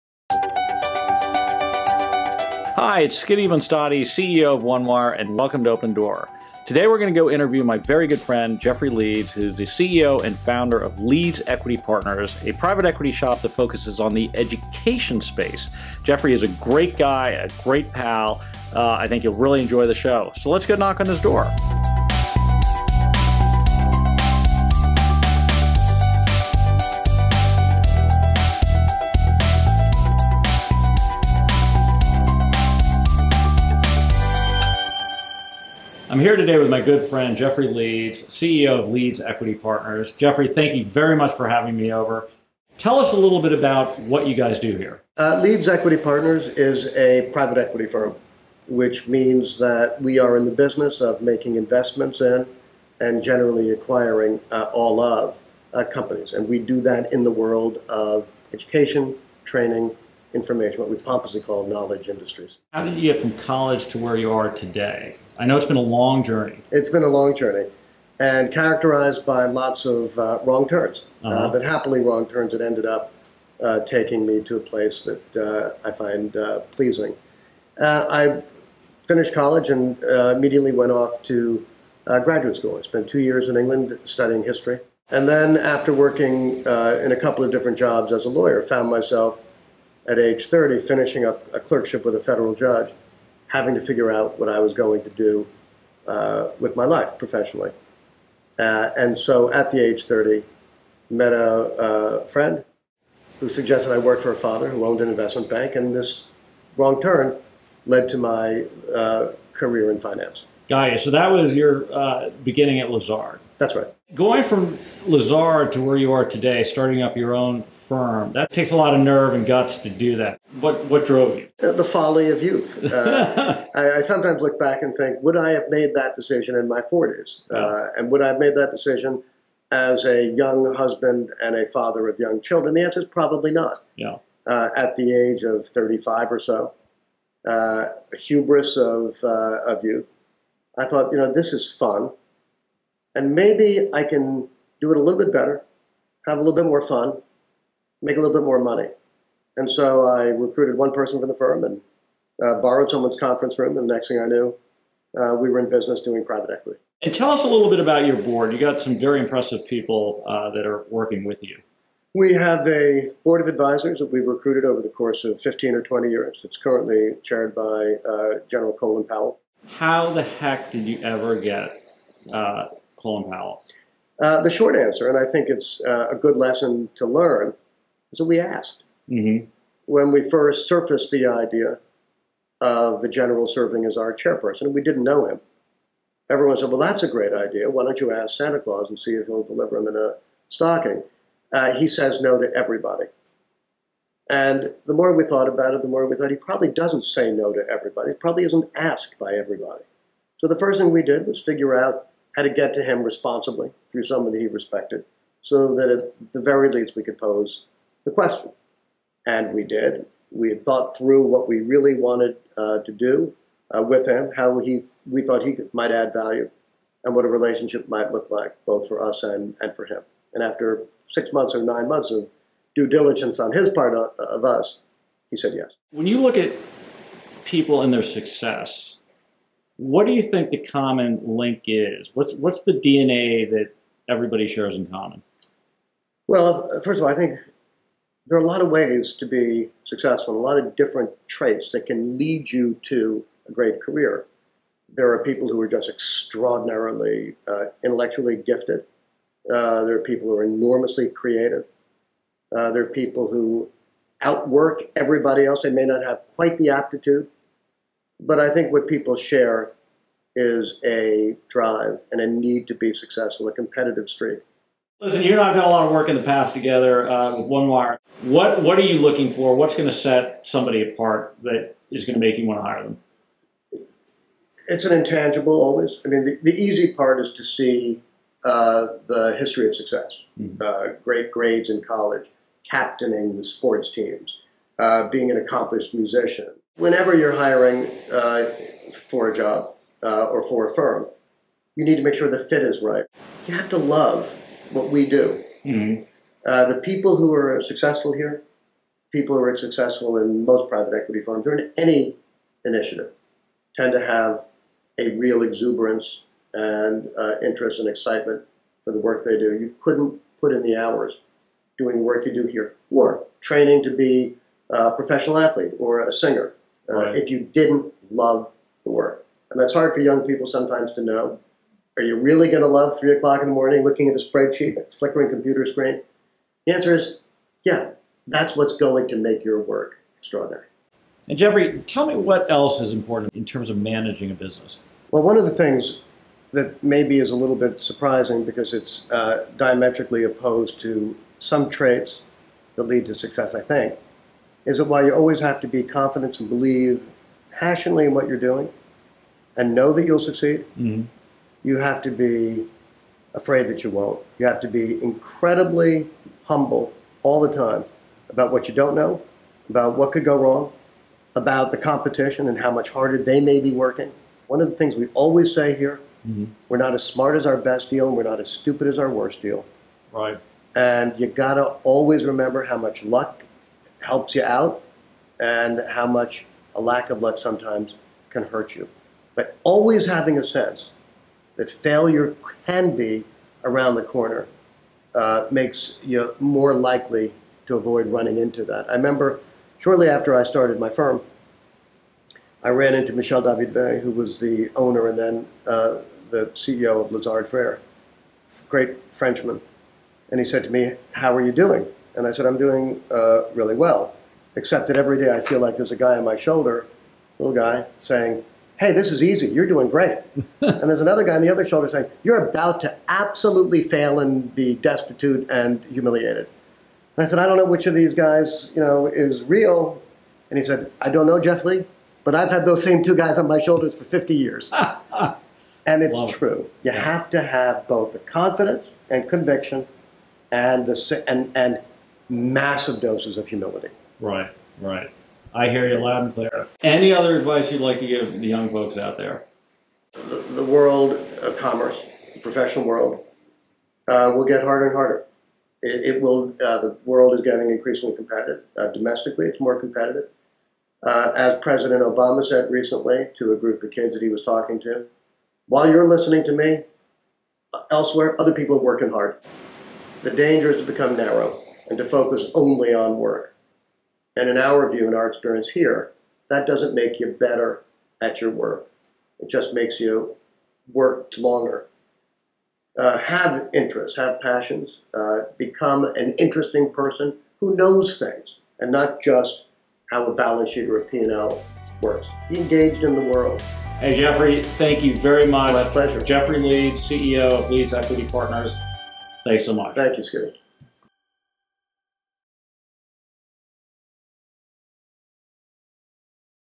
Jeffrey Leeds, President & Co-founder - Leeds Equity Partners
Jeffrey Leeds began his career in law, working in the corporate department of a New York-based law firm. After finishing up a clerkship for a federal judge, he found himself at a crossroads, needing to decide what he wanted to do with the rest of his professional life. It was at this time he was introduced to the world of finance. Jeffrey started working in mergers and acquisitions and corporate finance at Lazard Frères & Co. After that, he took a leap of faith and co-founded his own private equity firm, Leeds Equity Partners, which focuses on the education, information services, and training industries. The firm's advisory board now includes such prominent members as General Colin Powell and Timothy Shriver, Ph.D.